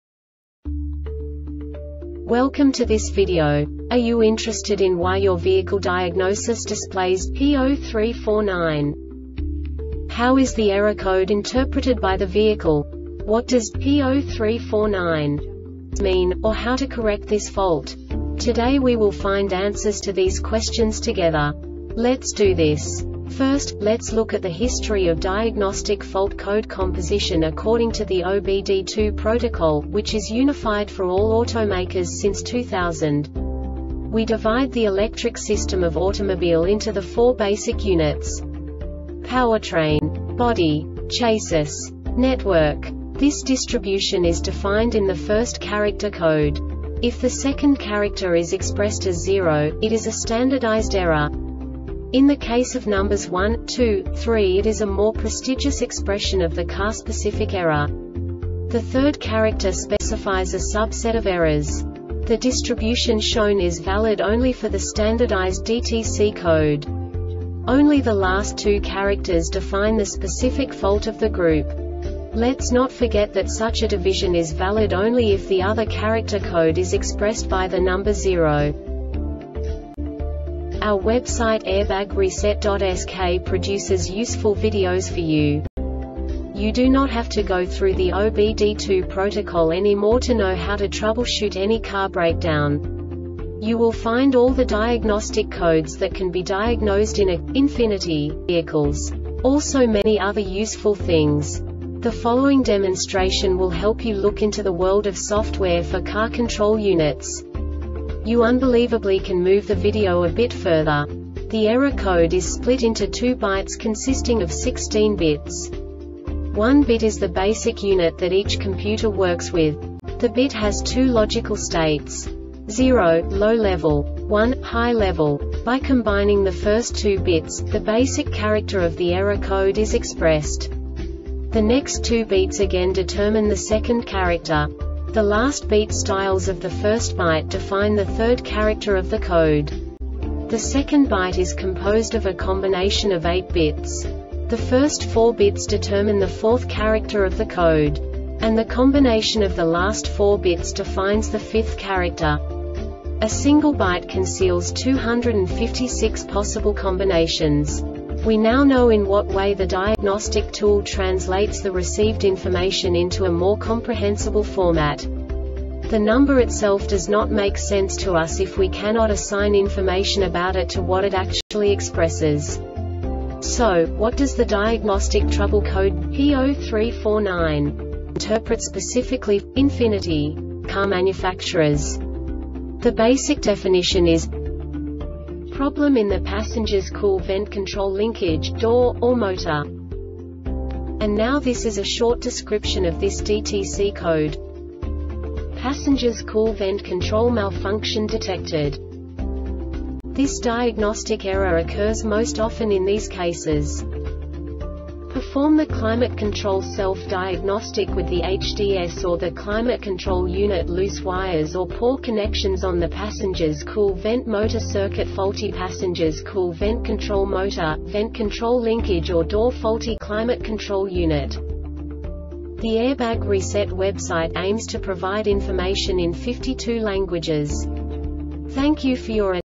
DTC Ford P0349 Short Explanation
Contents:
0:21 Basic DTC analysis according to OBD2 protocol standard.
1:48 Insight into programming
2:58 A diagnostically understandable description of the Diagnostic Trouble Code
3:05 Basic error definition

Make:
Ford
Code:
P0349
Definition:
Problem in the Passenger's Cool Vent Control Linkage, Door, or Motor
Description:
Passenger's Cool Vent Control malfunction detected.
Cause:
 Perform the climate control self-diagnostic with the HDS or the climate control unit Loose wires or poor connections on the passenger's cool vent motor circuit Faulty passenger's cool vent control motor,  vent control linkage or door Faulty climate control unit